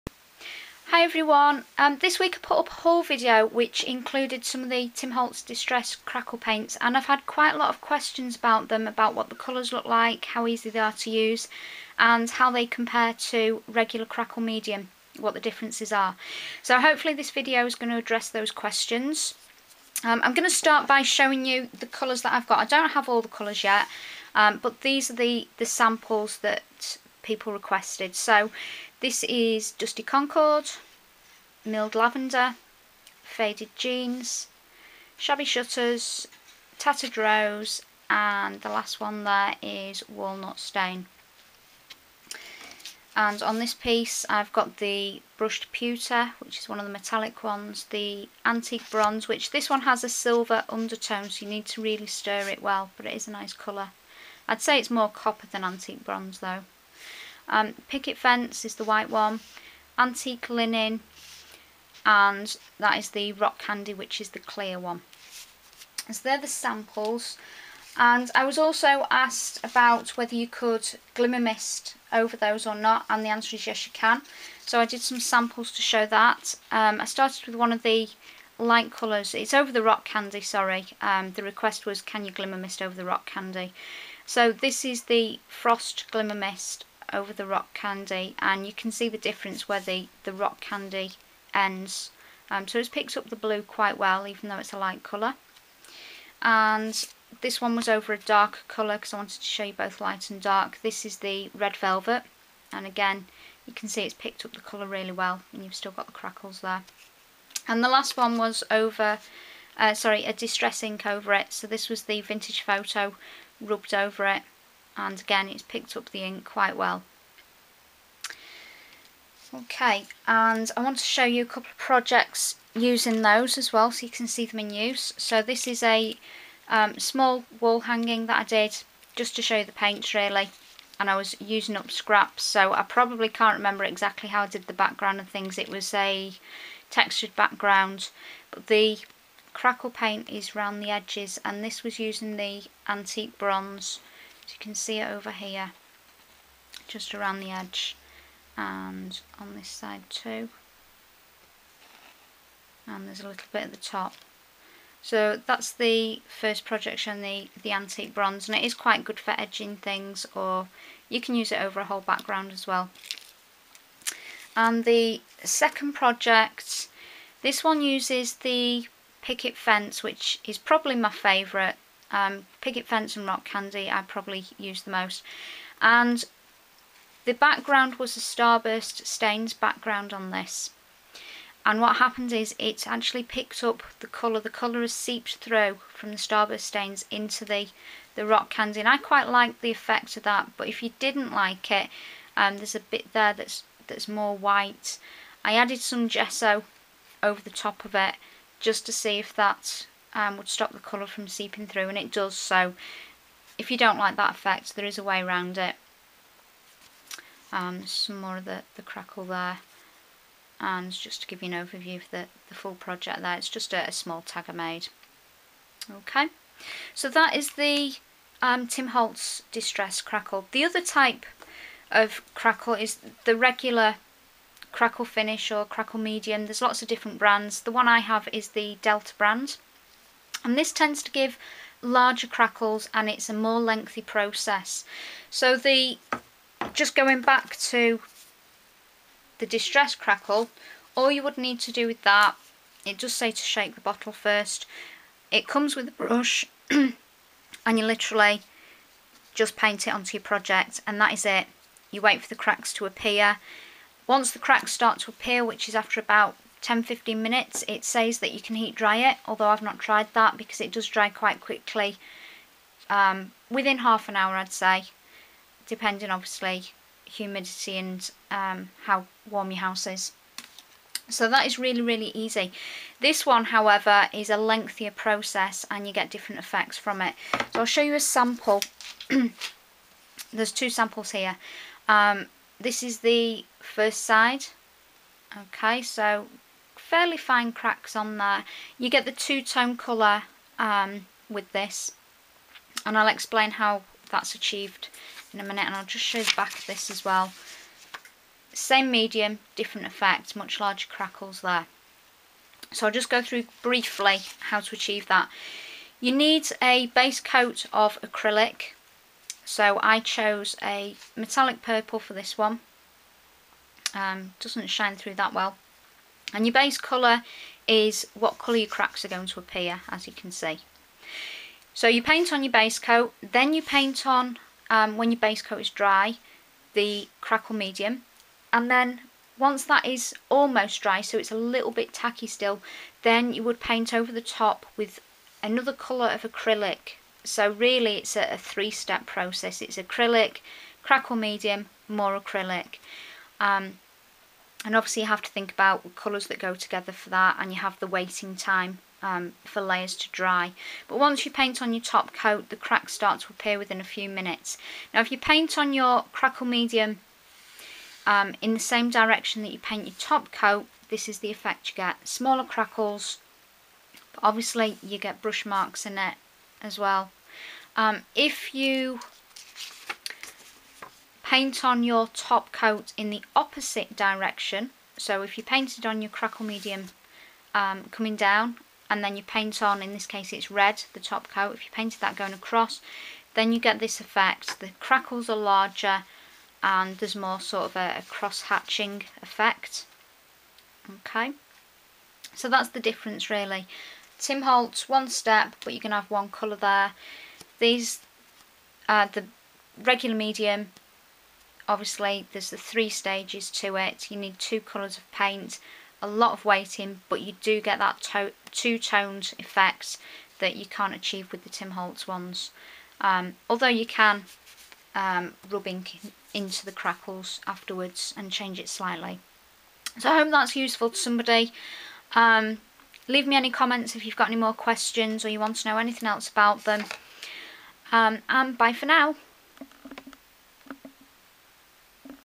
Tim Holtz Distress Crackle Paint Versus Traditional Crackle Medium - With samples & projects
Following a large number of requests about the Tim Holtz Distress Crackle Paints, I am posting a video response to show the colours, how to use them, and how they compare to regular crackle medium.